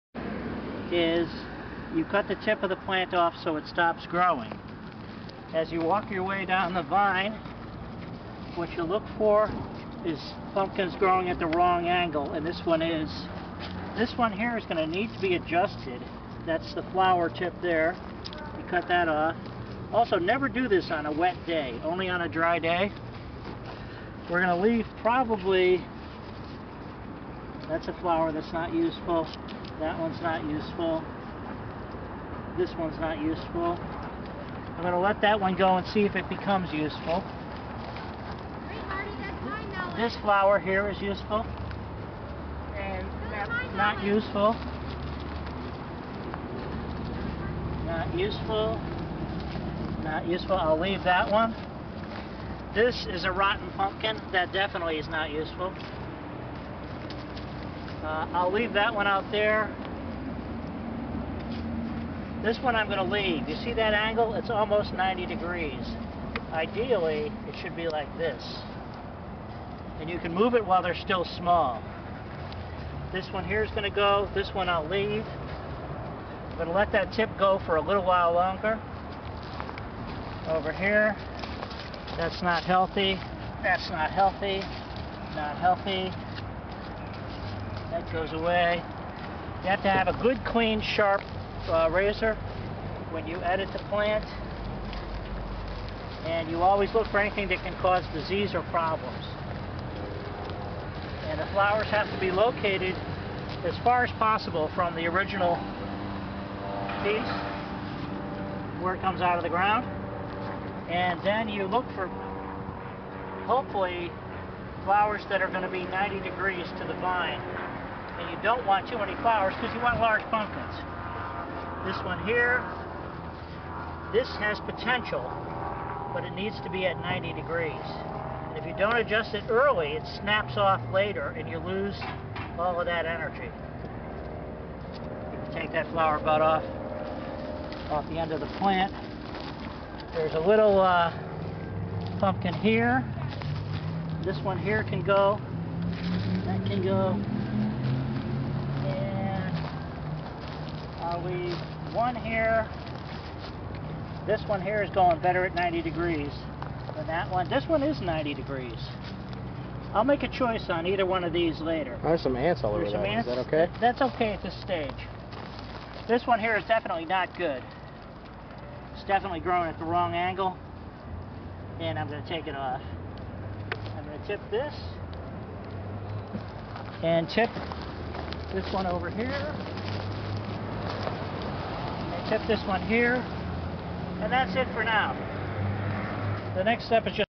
Marty Growing Giant Pumpkins(part 3)
Time to prune the vines,tips,flowers and bad bud sets. More water;more fertilizer!